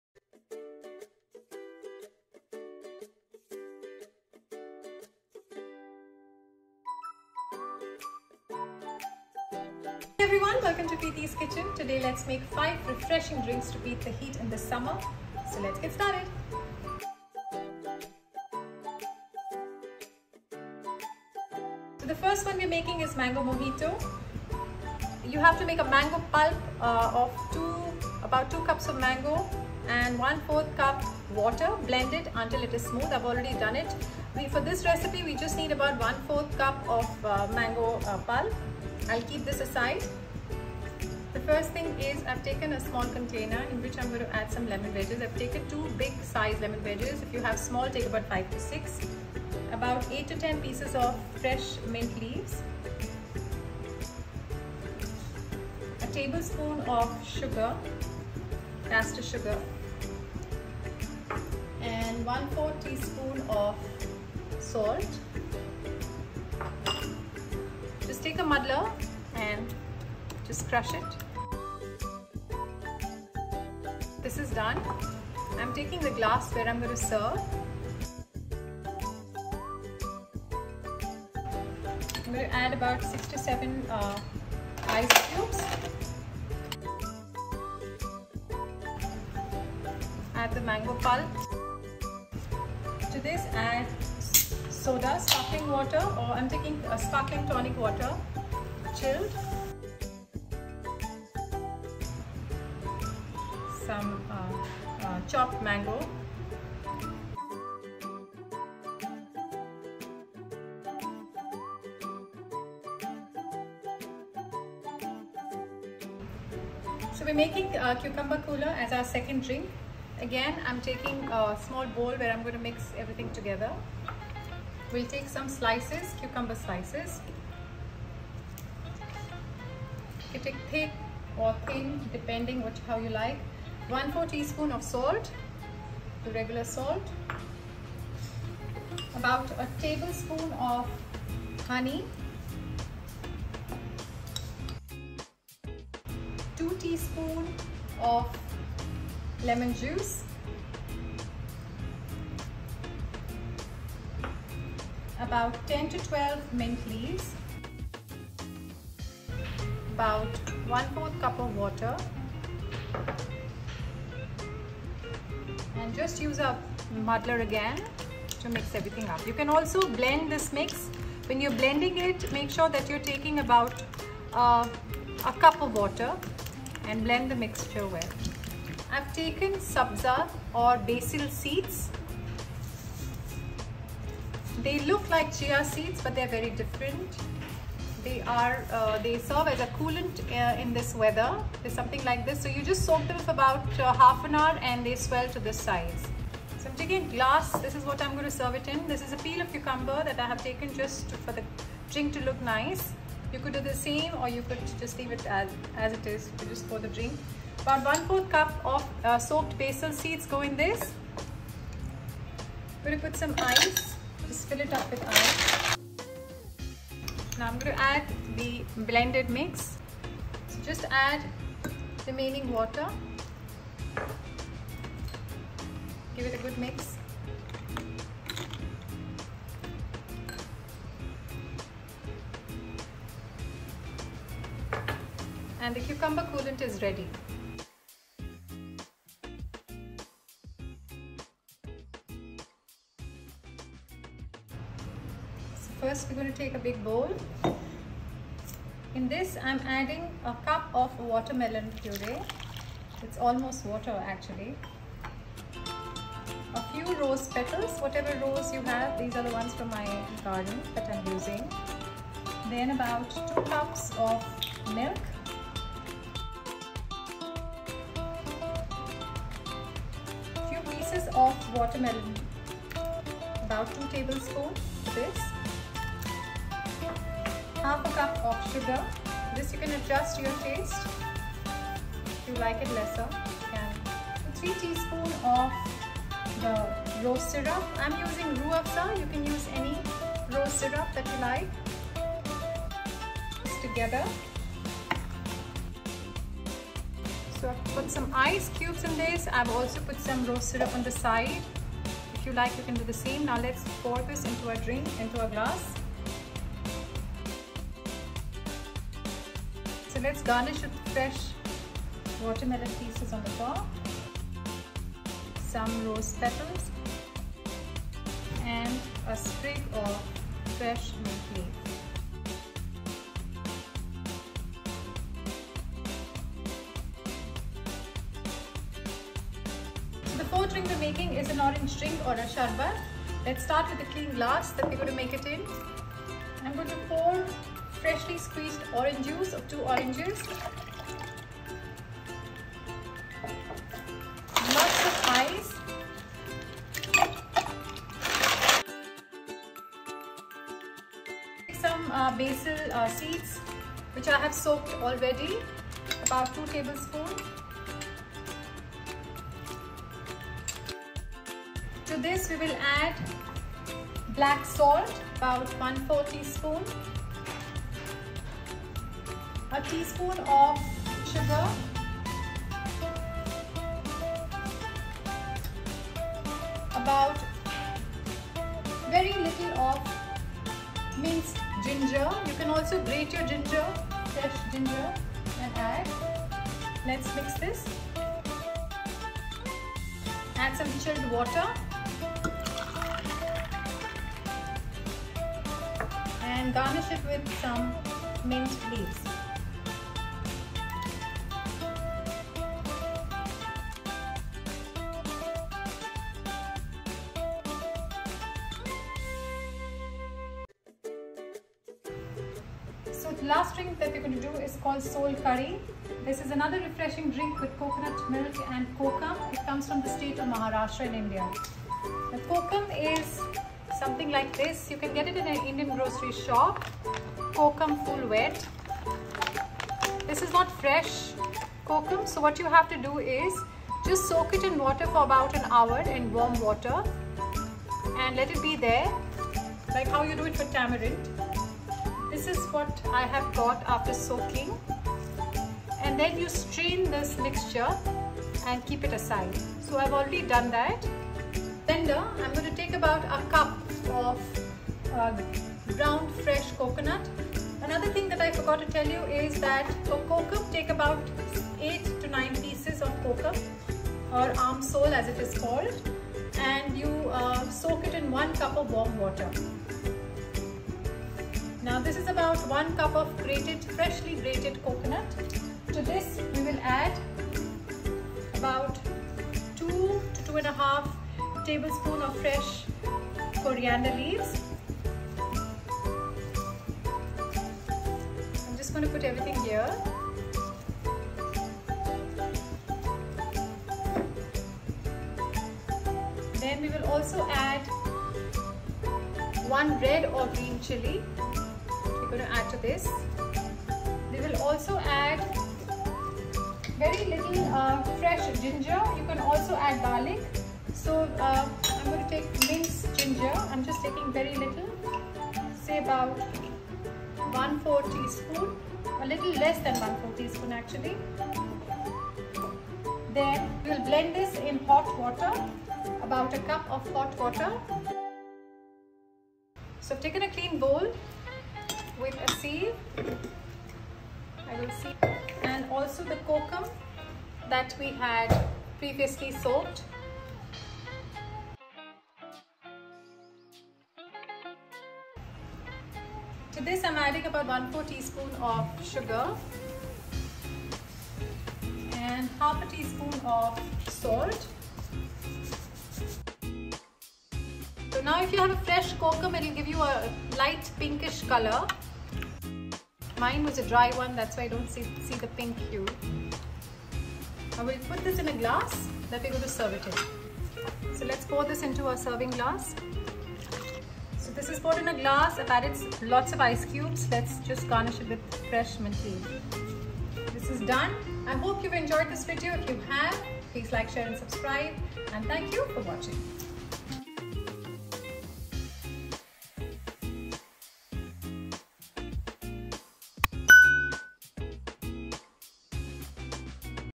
5 Refreshing Non- Alcoholic Summer Drinks
Light, refreshing and cooling non-alcoholic drinks to beat the heat this summer

Ingredients:

Mango Mojito:
2 lemon wedges
8-10 mint leaves
1 tbsp sugar
 1/4 tsp salt
Ice cubes
1/4 cup mango purée/ pulp
Sparkling tonic water
Garnish with mango slices & a sprig of mint

Cucumber Coolant:
Cucumber slices
 1/4 tsp salt
1 tbsp honey
2 tsp lemon juice
10-12 mint leaves
1/4 cup water
1/4 cup soaked basil seeds
Ice cubes

Rose & Watermelon Drink/ Sherbet:
1 cup Watermelon purée
Rose petals
2 cups milk
3 tbsp of Watermelon slices
1/2 cup sugar
3 tbsp rose syrup
Ice cubes
Garnish with watermelon slices, rose petals &
A sprig of mint

Orange Basil Drink:
Juice of 2 oranges
2 tbsp Lemon Juice
Ice cubes
2 tbsp basil seeds
1 tsp sugar/ palm sugar
1/4 tsp black salt
Minced ginger
Water
Garnish with mint leaves

Maharashtrian Sol Kadhi:
1 cup grated coconut
2-2  1/2 tbsp coriander leaves
1 red/ green chilli
1/4 tsp minced ginger
1 cup hot water
Soaked Kokum water
1/4 tsp sugar
1/2 tsp salt
Ice cubes